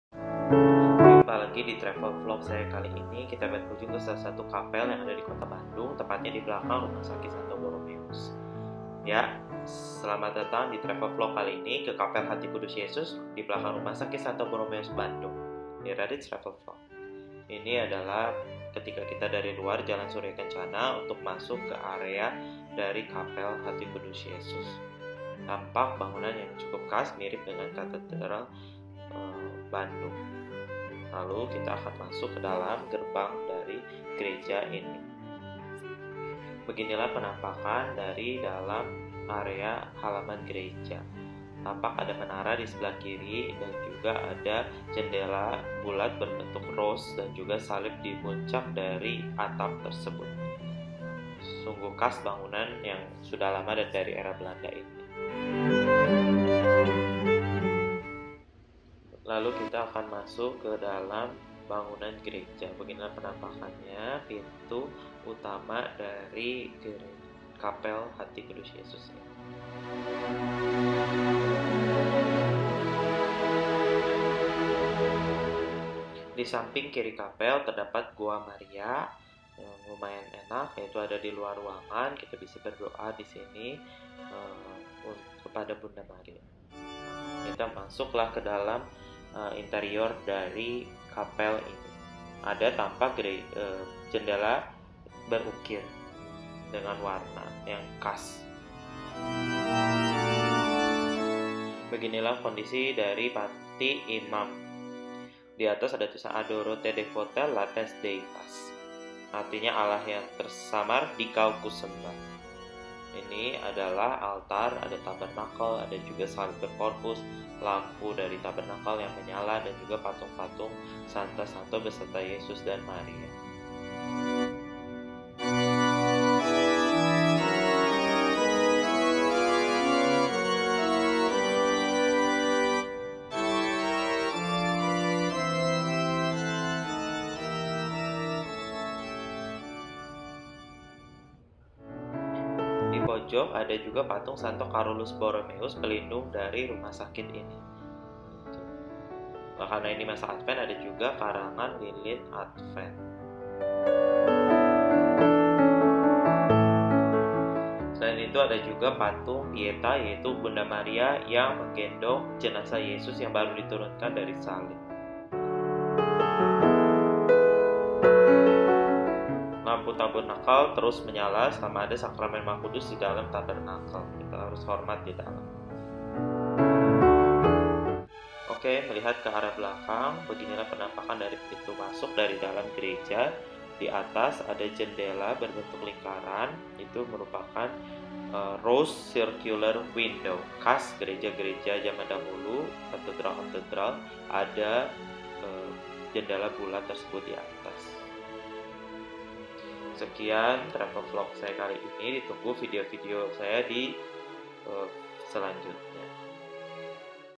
KAPEL HATI KUDUS YESUS - BORROMEUS - BANDUNG - Radit's Travel Vlog
Di travel vlog kali ini, saya berkunjung ke salah satu kapel yang sudah ada sejak dahulu, yaitu kapel Hati Kudus Yesus, RS St. Borromeus, Bandung. Selamat menikmati. Terima kasih.